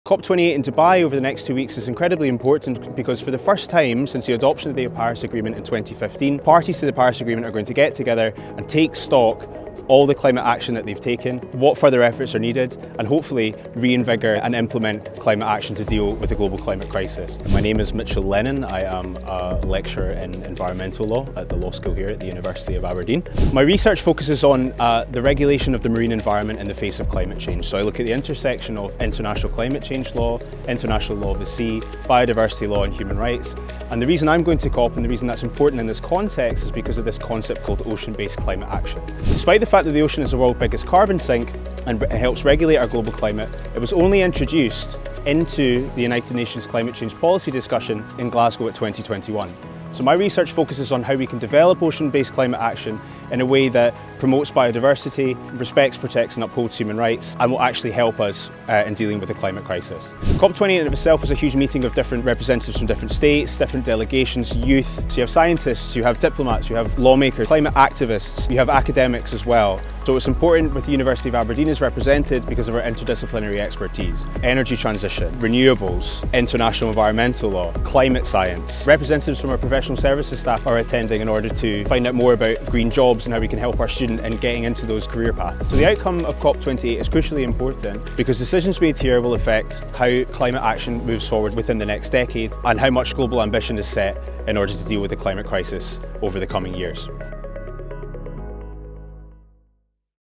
COP28 - crucial time for climate crisis
One of our COP28 attendees, Dr Mitchell Lennan, explains why COP28 is so important in the